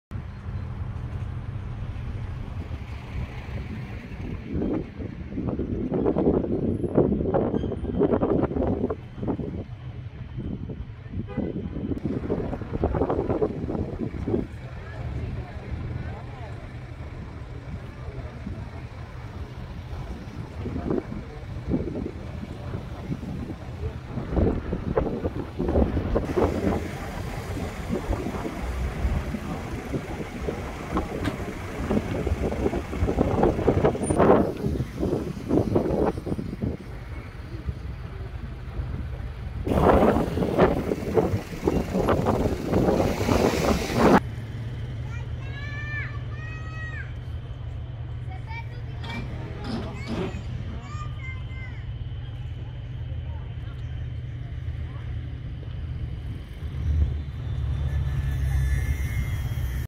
Το Azamara Quest στο λιμάνι του Αγ. Νικολάου
Το κρουαζιερόπλοιο Azamara Quest στο λιμάνι του Αγίου Νικολάου Κρήτης
9 Σεπτεμβρίου 2021